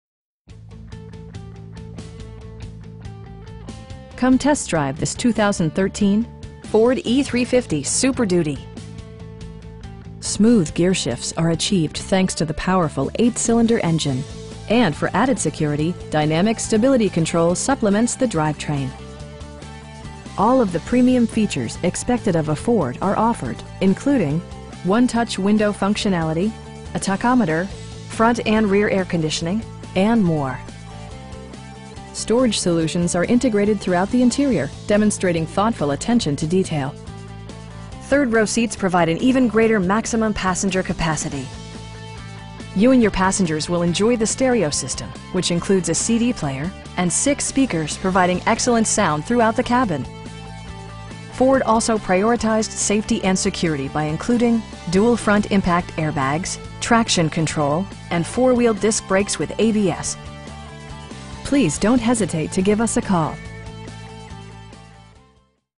2013 Ford E-350 Super Duty XLT in Caledonia, MI 49316
Broadmoor Motor Group

Come test drive this 2013 Ford E-350 Super Duty. Smooth gearshifts are achieved thanks to the powerful 8 cylinder engine, and for added security, dynamic Stability Control supplements the drivetrain. All of the premium features expected of a Ford are offered, including: 1-touch window functionality, a tachometer, front bucket seats, front and rear air conditioning, tilt steering wheel, cruise control, and more. Storage solutions are integrated throughout the interior, demonstrating thoughtful attention to detail. Third row seats provide an even greater maximum passenger capacity. You and your passengers will enjoy the stereo system, which includes a CD player, and 6 speakers, providing excellent sound throughout the cabin. Ford also prioritized safety and security by including: dual front impact airbags, traction control, and 4 wheel disc brakes with ABS. We know that you have high expectations, and we enjoy the challenge of meeting and exceeding them! Please don't hesitate to give us a call.